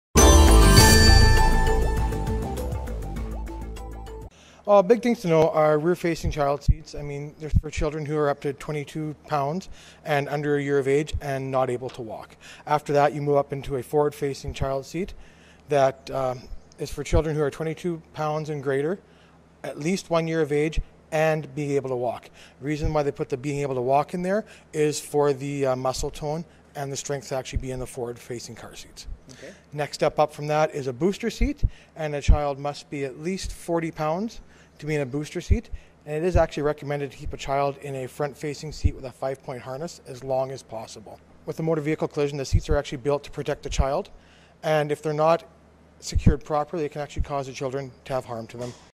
Car Seat Safety
Humboldt and District Ambulance paramedic Shaun Parfitt gives some car seat safety tips.